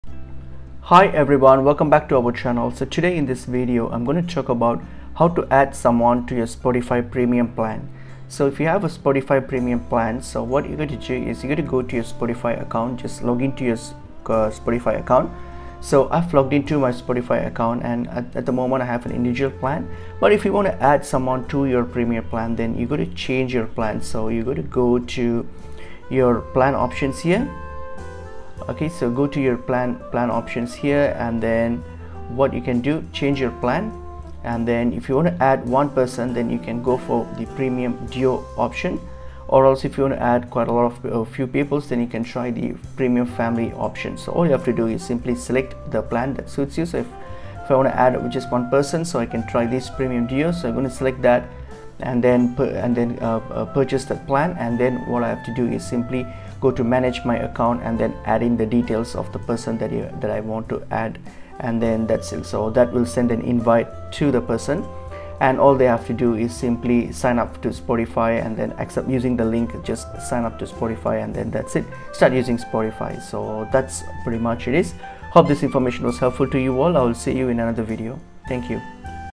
How to add someone to your Spotify premium plan (Best Method)
Discover the simple process of adding someone to your Spotify premium plan in this informative video. Whether you want to split the cost with a loved one or share the perks with a friend, we've got you covered with clear and concise instructions. Watch now and start enjoying premium features together!